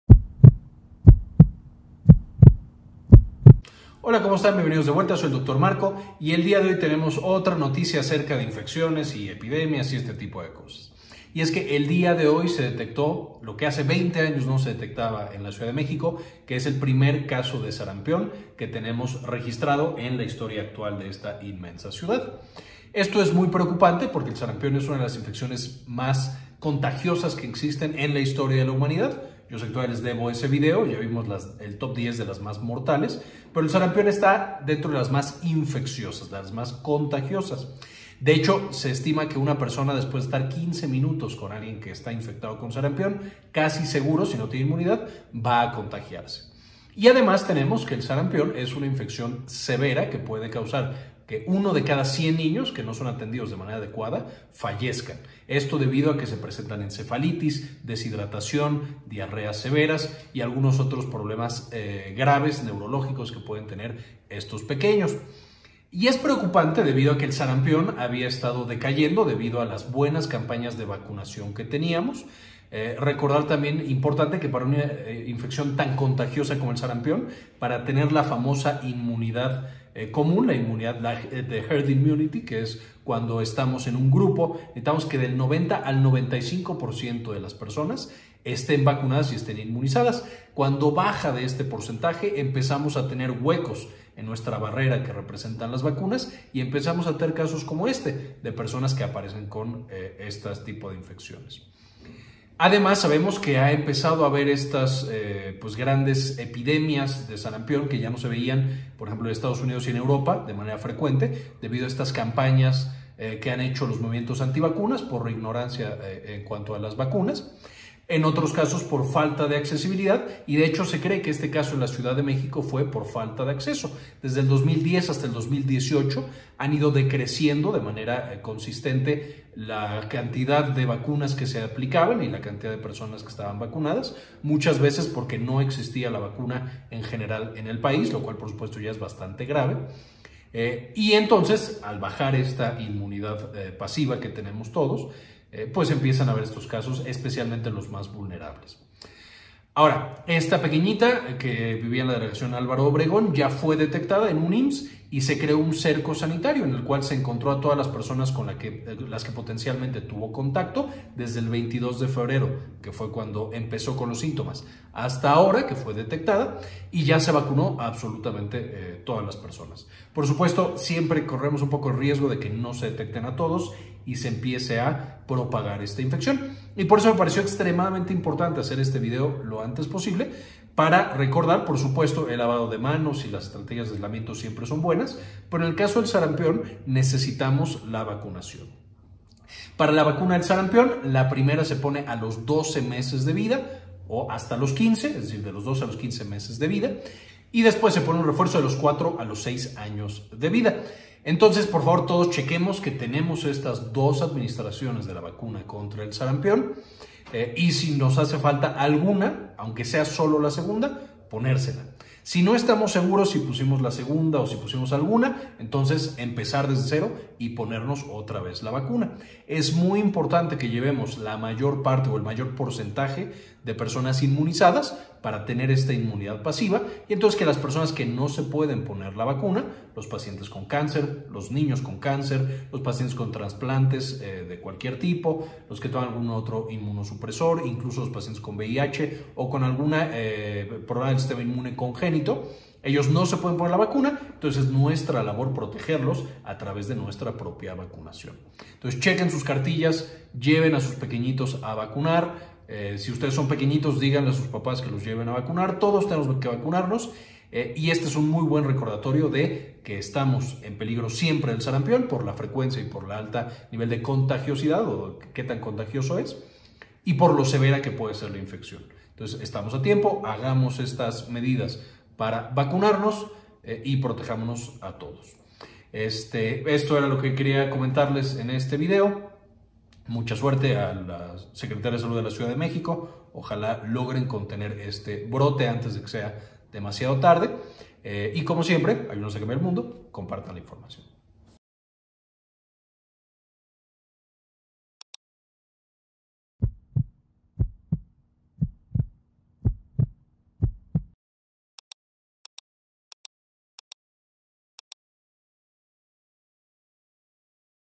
Sarampión vuelve a CDMX después de 20 años
El día de hoy hablamos del brote de sarampión que ocurrió el día de hoy en la Ciudad de México, comentamos las consecuencias de esta infección y qué hacer para protegernos y a nuestra familia.